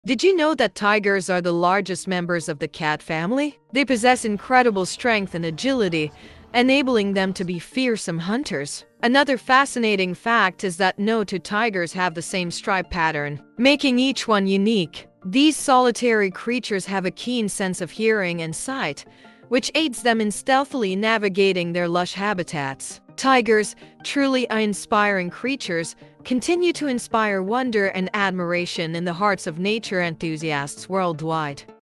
Unveiling the Mighty Tiger: Fascinating Facts You Didn't Know
In this captivating video, we dive deep into the world of the majestic tiger. Join us as we unveil a treasure trove of fascinating facts that will leave you amazed. From their awe-inspiring strength and agility to the mysteries behind their unique stripes, prepare to discover a side of tigers you never knew existed. Explore their incredible life cycle, learn about their hunting techniques, and understand the vital role they play in maintaining ecosystem balance. Get ready to be enthralled by the secrets of the mighty tiger in this eye-opening exploration. Don't miss out on this wild journey of discovery!